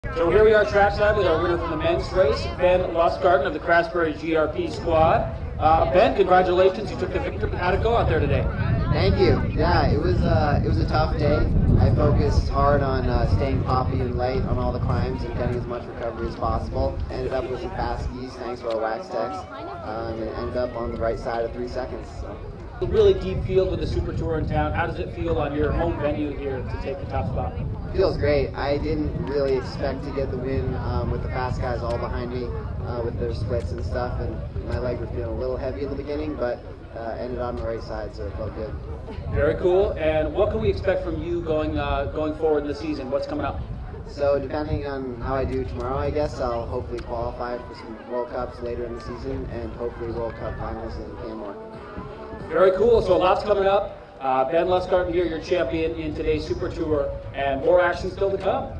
2020 - Lost Nation Cup: Day 2 winner, Ben Lustgarten - FS 10k interval start
David McCahill interviews Craftsbury Green Racing Project's Ben Lustgarten following his win in the men's 10k FS interval start in Day 2 of the LostNationCup.